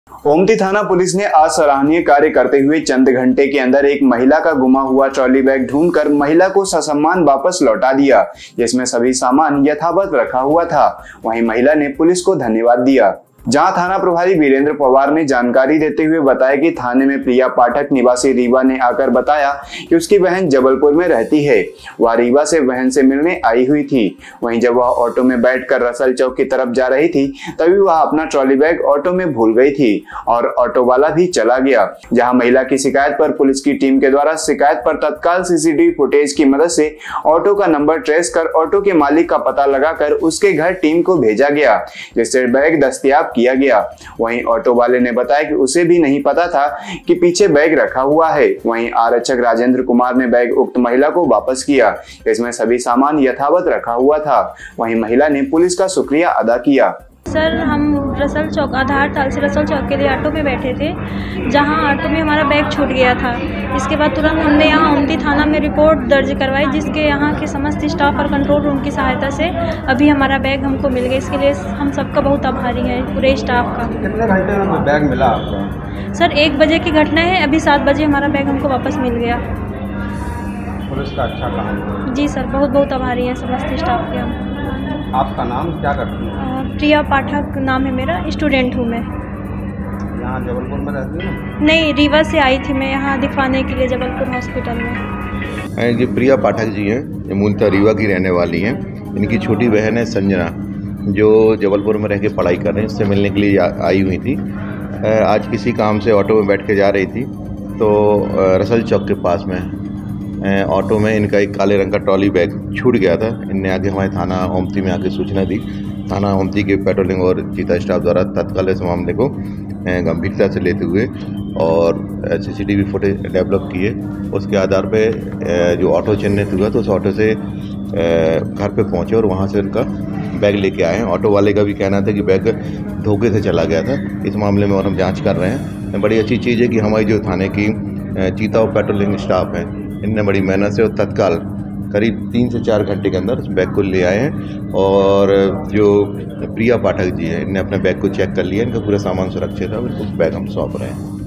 जबलपुर : ओमती थाना पुलिस ने पेश की मिसाल, ऑटो में छुटा महिला का बैग चंद घँटों में ढूंढकर किया वापिस
ओमती थाना पुलिस आज सराहनीय कार्य करते हुए चंद घंटे के अंदर एक महिला का गुमा हुआ ट्राली बैग ढूंढकर महिला को ससम्मान वापिस लौटा दिया,जहाँ थाना प्रभारी वीरेंद्र सिंह पवार ने जानकरी देते हुए बताया की थाने में प्रिया पाठक निवासी रीवा ने आकर बताया की उसकी बहन जबलपुर में रहती है वह रीवा से बहन से मिलने के लिए आई हुई थी वही जब वह ऑटो में बैठकर रसल चौक तरफ जा रही थी तब वह अपना ट्राली बैग ऑटो में भूल गयी और ऑटो वाला भी चला गया,जहाँ महिला की शिकायत पर पुलिस टीम के द्वारा शिकायत पर तत्काल सीसीटीवी फुटेज की मदद से ऑटो का नंबर ट्रेस कर ऑटो मालिक का पता लगाकर उसके घर टीम को भेजा गया,जिससे बैग दस्तयाब किया गया,वही ऑटो वाले ने बताया कि उसे भी नही पता था कि पीछे बैग रखा हुआ,वही  आरक्षक राजेंद्र कुमार ने बैग उक्त महिला को वापिस किया,जिसमे सभी सामान यथावत रखा हुआ था,वही महिला ने पुलिस का शुक्रियादा किया।